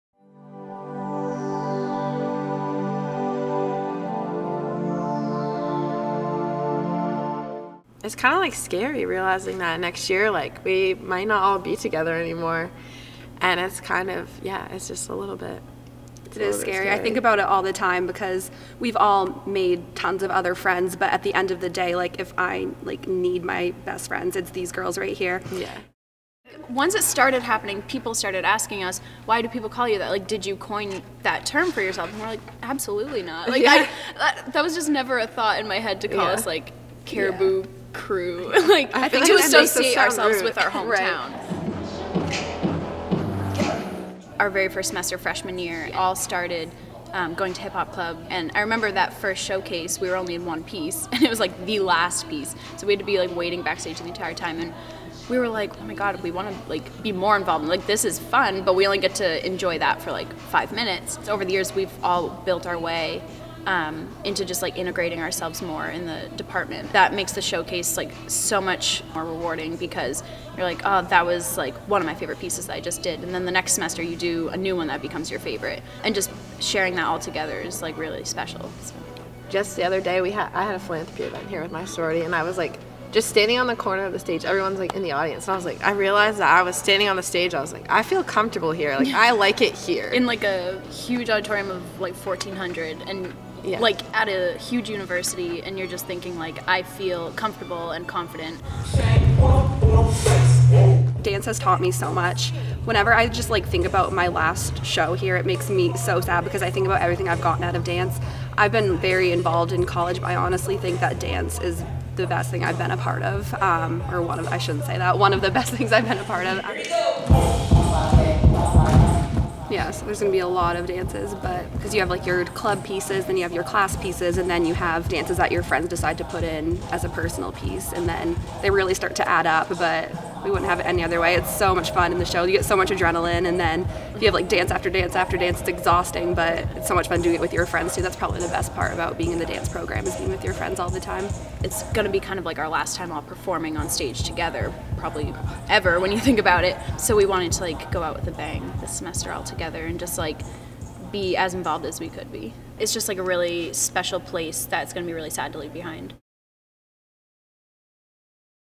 The Caribou Crew
Three best friends from Caribou Maine share their passion for dance in what will be their final performance together before graduating.